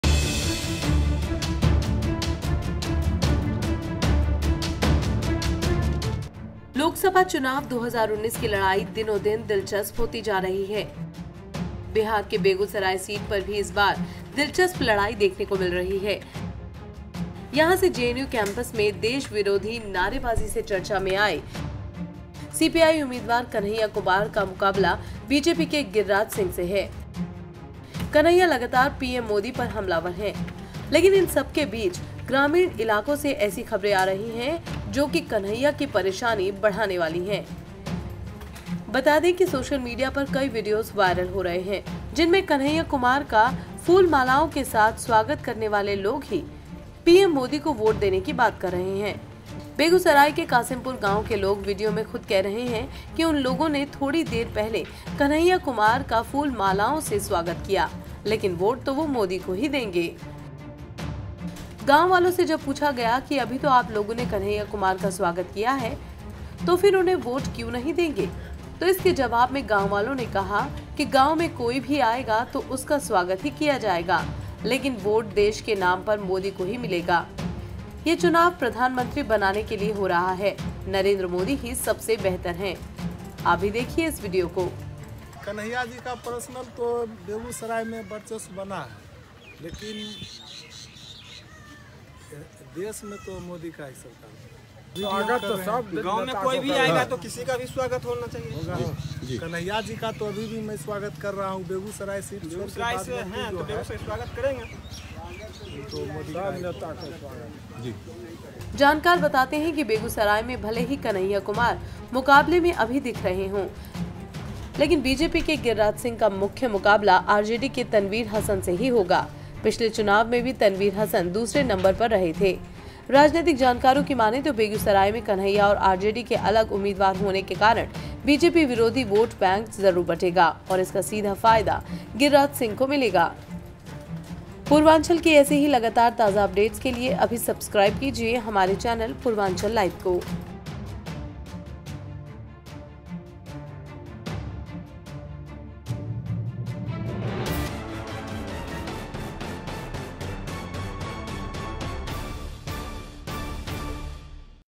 बेगूसराय में कन्हैया की राह मुश्किल, स्वागत करने वाले भी बोले- वोट तो मोदी को ही देंगे!
सोशल मीडिया पर कई वीडियो वायरल हो रहा है जिसमें कन्हैया कुमार का फूल-मालाओं के साथ स्वागत करने वाले लोग ही वोट पीएम मोदी को देने की बात कर रहे हैं।देखिए इस वीडियो में...